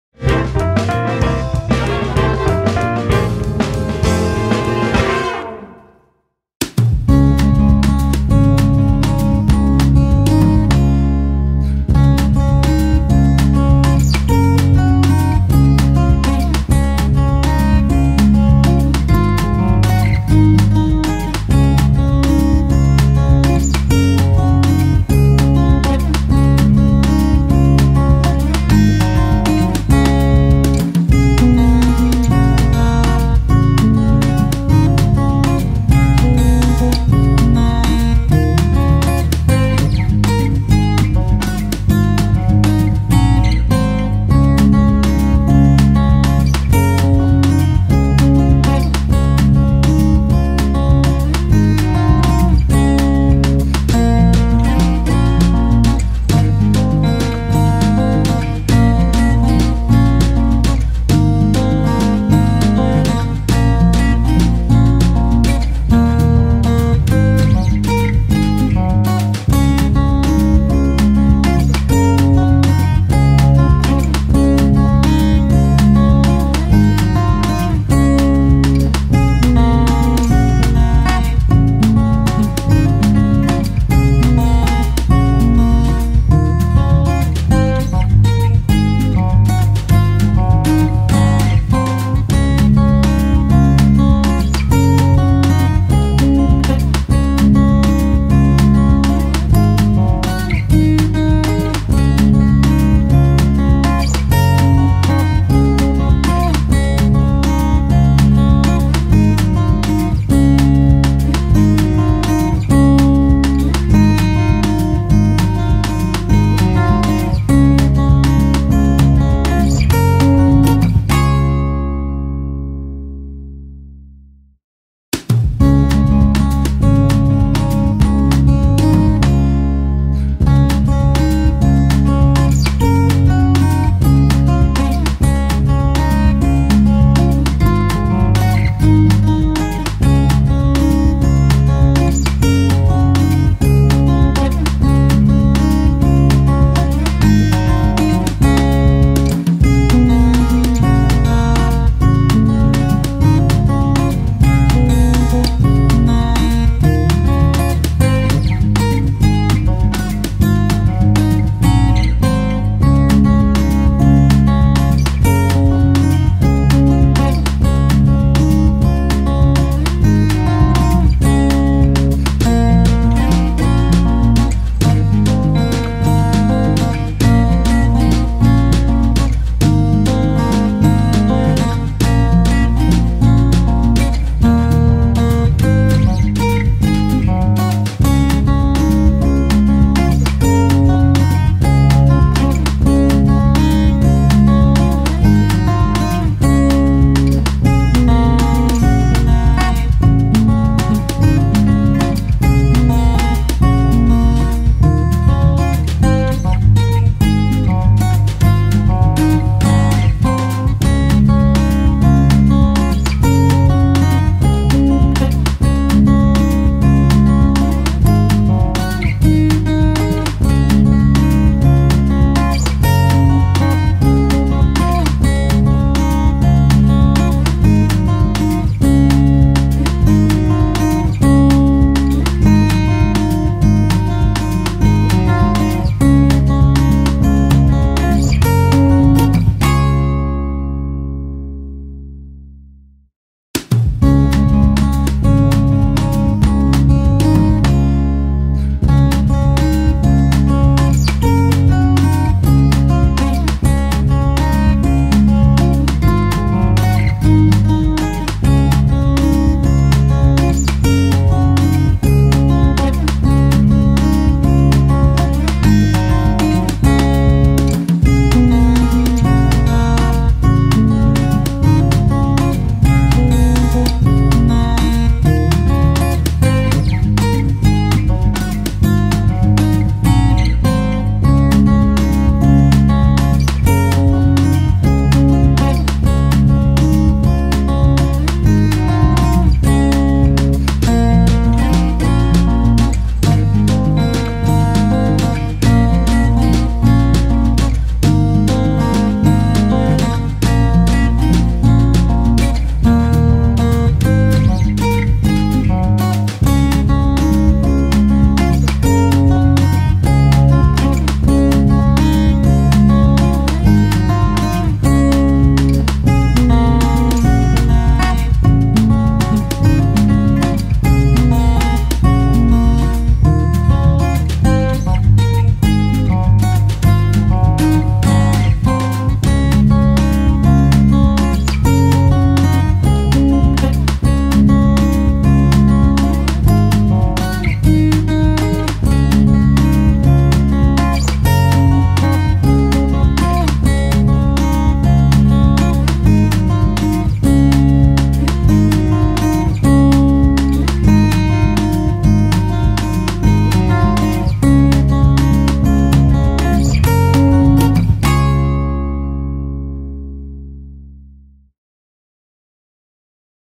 Poseidon Timelapse - Color Comps
We recorded the process of finishing a full character illustration in Photoshop. This is part 2: color comps.